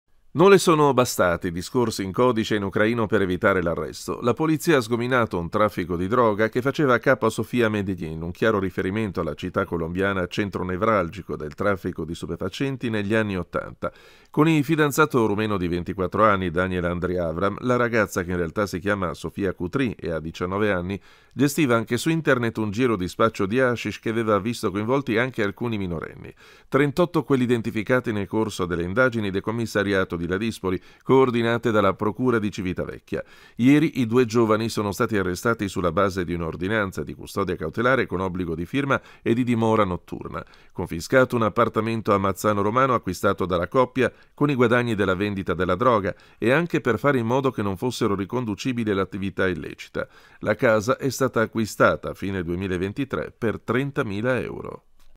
Polizia, arrestata "Sofia Medellin" con il fidanzato
Non le sono bastati i discorsi in codice e in ucraino per evitare l'arresto. La polizia ha sgominato un traffico di droga che faceva capo a «Sofia Medellin», un chiaro riferimento alla città colombiana centro nevralgico del traffico di sostanze stupefacenti negli anni Ottanta. Con il fidanzato rumeno di 24 anni, Daniel Andrei Avram, la ragazza, che in realtà si chiama Sofia Cutrì ed ha 19 anni, gestiva anche su internet un giro di spaccio di hashish che aveva visto coinvolti anche alcuni minorenni. 38 quelli identificati nel corso delle indagini del commissariato di Ladispoli, coordinate dalla Procura di Civitavecchia. Ieri i due giovani sono stati arrestati sulla base di un’ordinanza di custodia cautelare con obbligo di firma e di dimora notturna. Confiscato un appartamento a Mazzano Romano acquistato dalla coppia con i guadagni della vendita della droga e anche per fare in modo che non fossero riconducibili all'attività illecita. La casa è stata acquistata a fine 2023 per 30mila euro.

DOVE SEGUIRCI